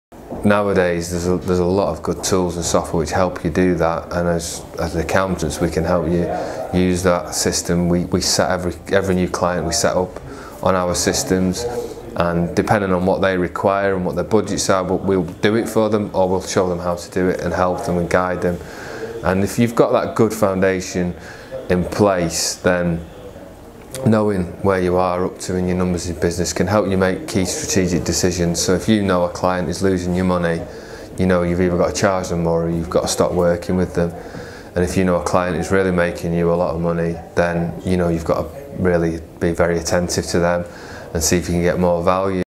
RJF How we can help at RJF especially with software
Running a business is hard enough as it is… Dealing with customers, clients, suppliers,

employees, it’s a juggling act even the most seasoned entrepreneurs find challenging.

The area that regularly drops by the wayside first with regards to attention is accounting and

finance. Business owners and CEOs often can fall into the trap of solely focusing on bringing on

clients.

Sales, sales, sales! After all; sales pay the bills right?

If money is coming in and the business is profitable what more is there to think about?!

Well, sales are definitely key, but how often do you sit down and really dig into the numbers? Can accounting software help make your life a lot easier?

RJF are here to help with all your accounting and business growth needs.